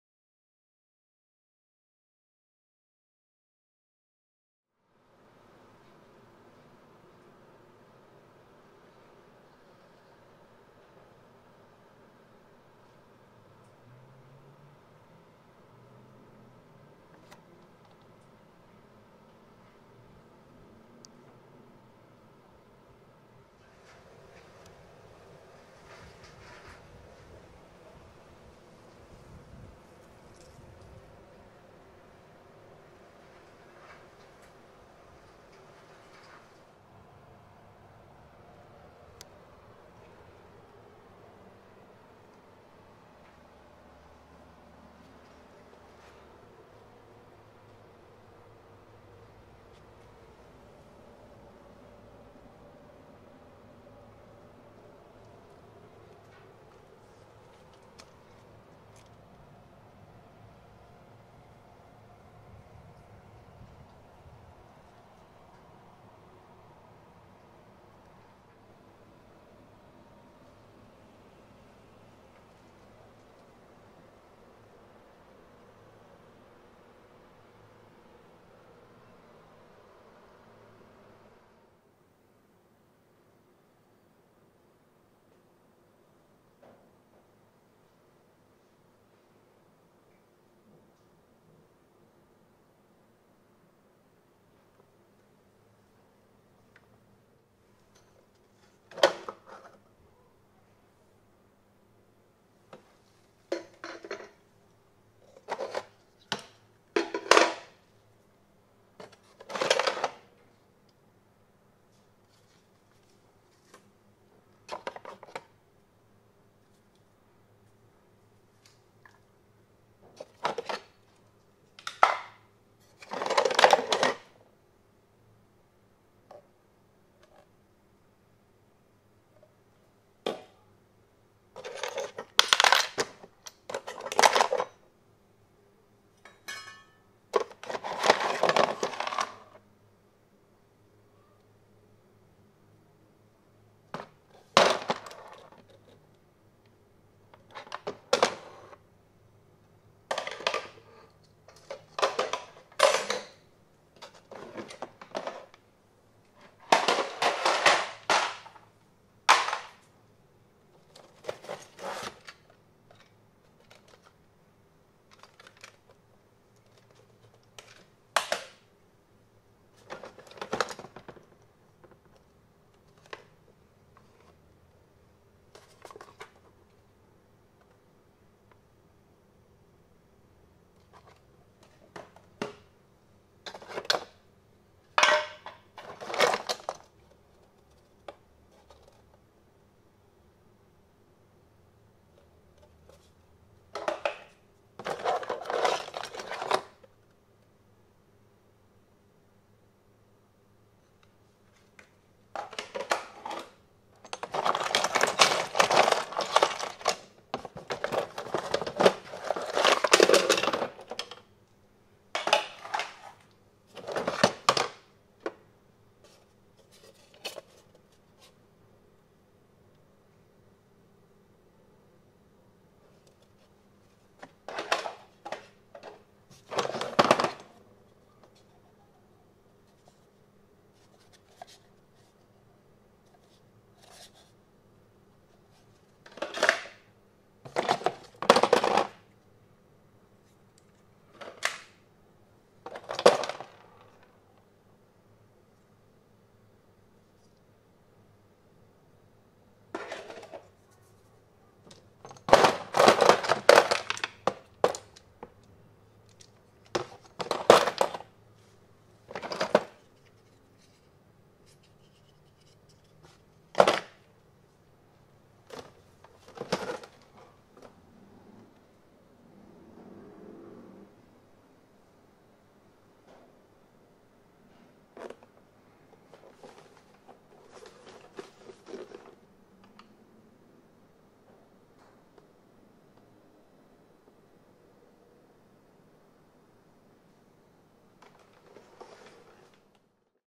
What Parts I kept from the E34
The interior was basically trashed and there wasn't anything still in good condition so I let it all go. Due to the low value of iron at the moment, it is actually costing me $120 to have the body picked up and recycled, not long ago they payed you to take it away. Not now!

Not sure anyone will ever want any of these parts- though the instrument cluster LCD's are free of dead pixels and work perfectly. Thats one of the few fixes I ever did on this car for my friend - that and repairing the alternator. He just never wanted to spend money on it from day one.

Someone has also removed the check engine light bulb in this car too. It will forever remain a mystery what it was complaining about. I have a carsoft 6.5 adapter on its way too but will play around with it on the e39 perhaps one day unless I come across another cheap e34 in way better condition than this one was in to give another chance at life.